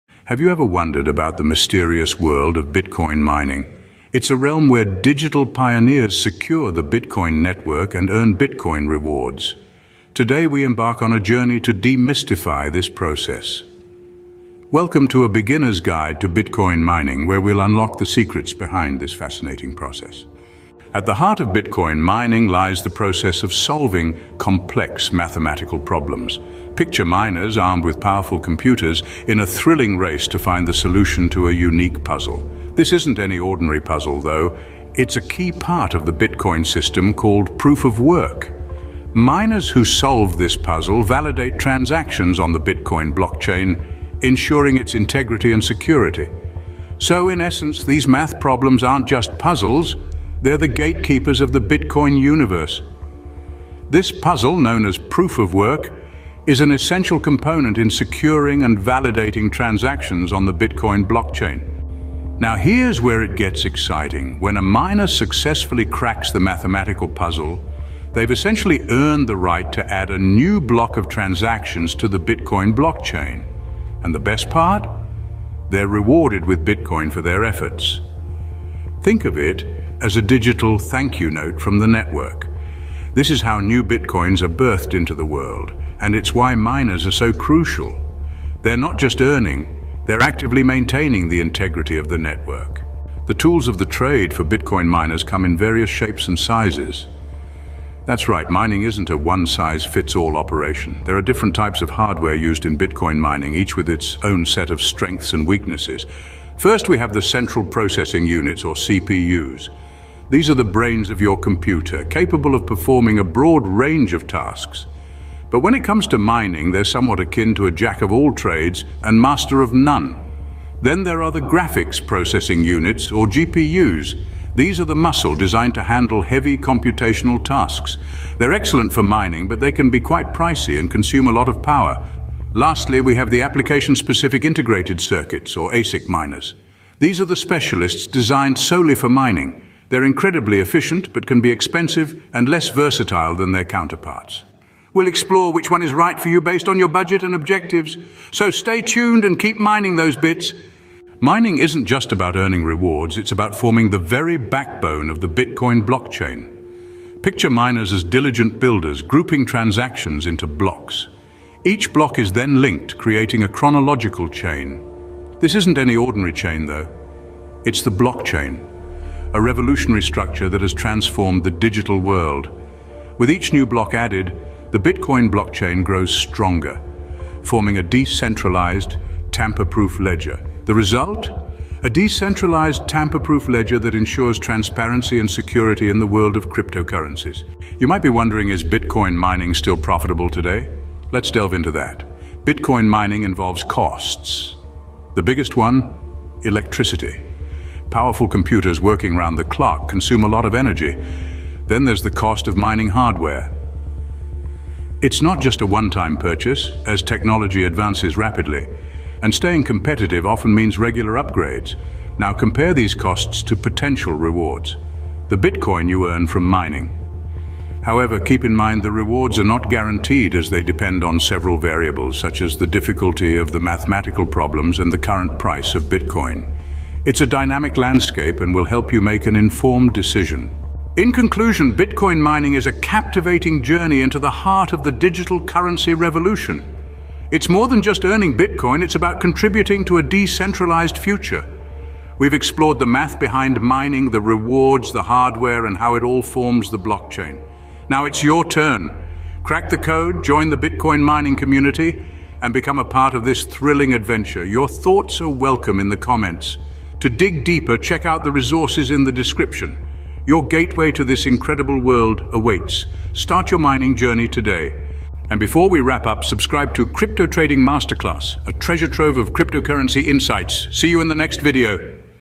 Cracking the Code: A Beginner's Guide to Bitcoin Mining - EP13 | Crypto Trading Masterclass
In this beginners guide video, we unravel the fascinating world of Bitcoin mining, demystifying its complex process, tools, and the lucrative rewards it holds. If you're intrigued by cryptocurrencies or considering diving into the mining world, then this video is perfect for you.

Join us as we break down the intricacies of Bitcoin mining in an easily understandable way, empowering you to grasp the fundamentals and get started on your own mining journey.

TIMESTAMPS:
00:00:00 Introduction to Bitcoin Mining
00:00:20 Solving Complex Math Problems
00:01:01 Earning Bitcoin for Your Efforts
00:01:32 CPUs, GPUs, and ASIC Miners
00:02:30 Forming the Blockchain Through Mining
00:03:07 Is Mining Profitable Today? Costs vs. Rewards
00:03:50 Conclusion and Call to Action
00:04:25 Subscribe to Our Channel